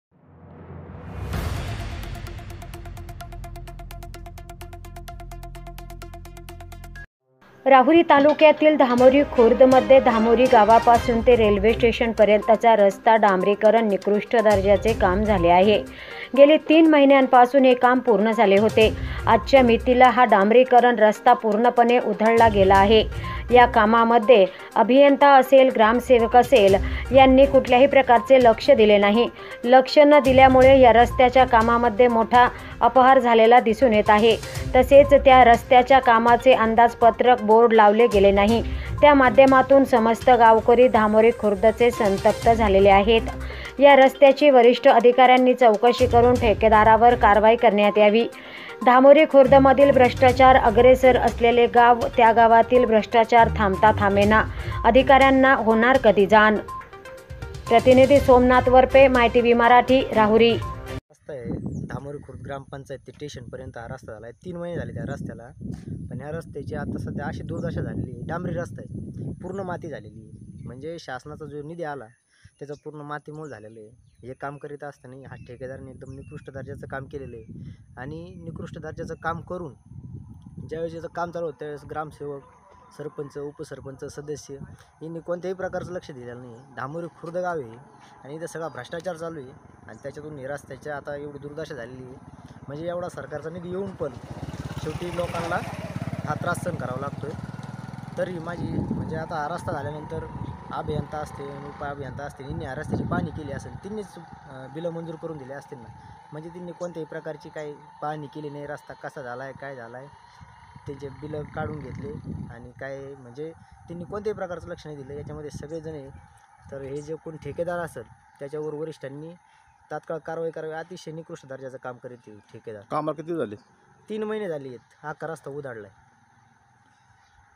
राहुरीतालुक्यातील धामोरी खुर्द मध्ये धामोरी गावापासून ते रेल्वे टेशन पर्यंत चा रस्ता उखडला
राहुरीतालुक्यातील धामोरी खुर्द मध्ये धामोरी गावापासून  ते रेल्वे टेशन पर्यंत चा रस्ता डांबरीकरण निकृष्ट दर्जाचे काम झाले आहे गेली तीन महिन्यापासून हे काम पूर्ण झाले होते आजच्या मी तिला हा डांबरीकरण रस्ता पूर्णपणे उधळला गेला आहे या कामांमध्ये अभियांता असेल ग्रामसेवक असेल यांनी कुठल्याही प्रकारचे लक्ष दिले नाही लक्ष न दिल्यामुळे या रस्त्याच्या कामांमध्ये मोठा अपहार झालेला दिसून येत आहे तसेच त्या रस्त्याच्या कामाचे अंदाज पत्र बोर्ड लावले गेले नाही त्या माध्यमातून समस्त गावकरी धामोरी खुर्द चे संतप्त झाले आहेत या रस्त्याची वरिष्ठ अधिकाऱ्यांनी चौकशी करून ठेकेदारावर कारवाई करण्यात यावी धामोरी खुर्द मधील भ्रष्टाचार अग्रेसर असलेले गाव त्या गावातील भ्रष्टाचार थांबता थांबेना अधिकाऱ्यांना होणार कधी  जान.